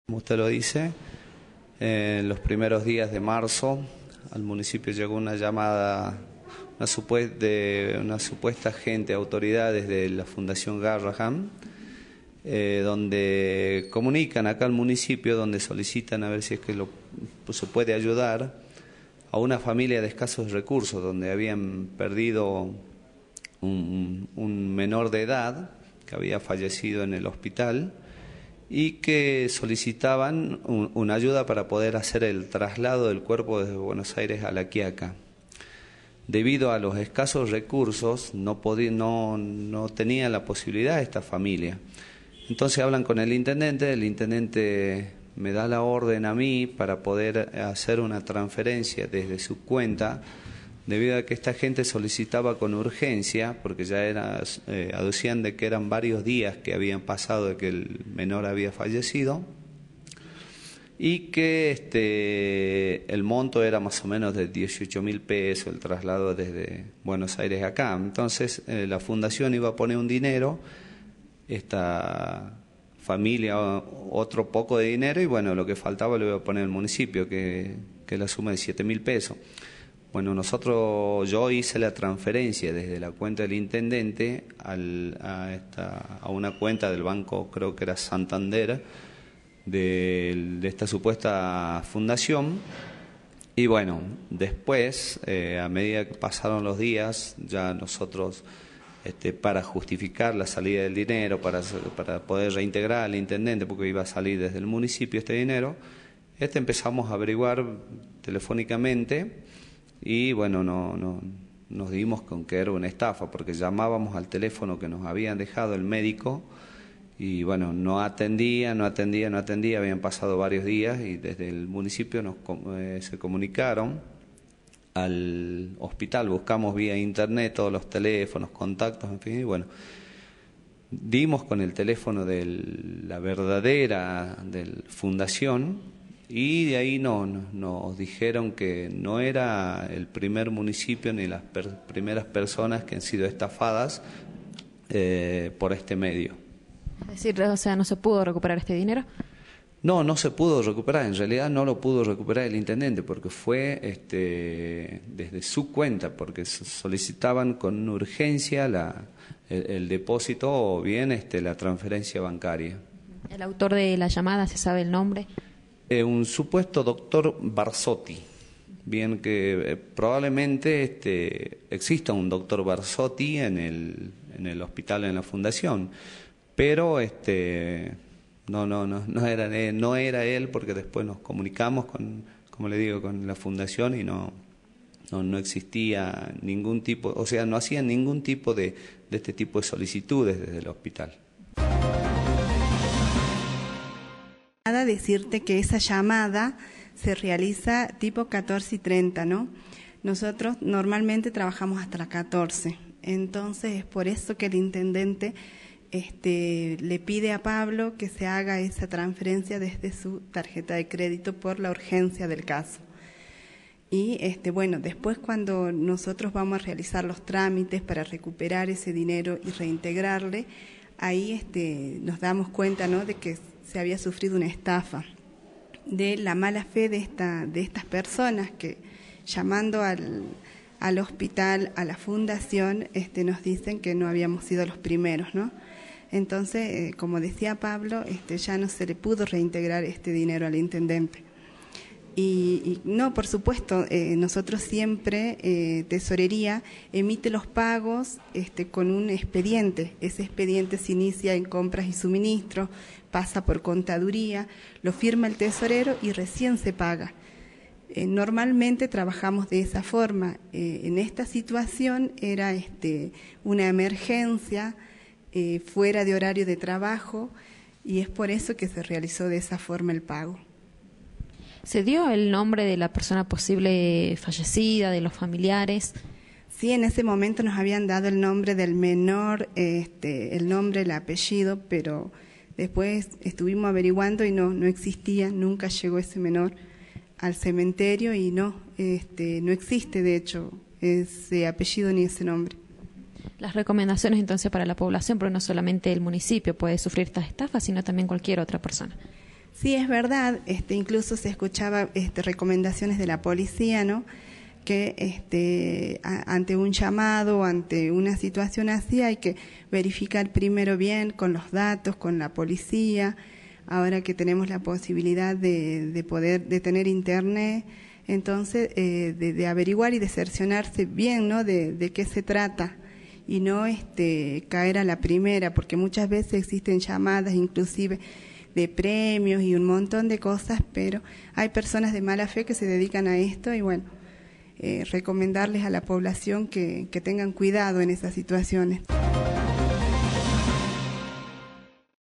LA QUIACA 22 JUNIO 2016 MUNICIPIO VICTIMA DEL ESTAFADOR SERIAL DE INTENDENTES
REPORTAJE A PABLO ALFARO Y JUSTINA CRUZ EMPLEADOS DE LA MUNICIPALIDAD DE LA QUIACA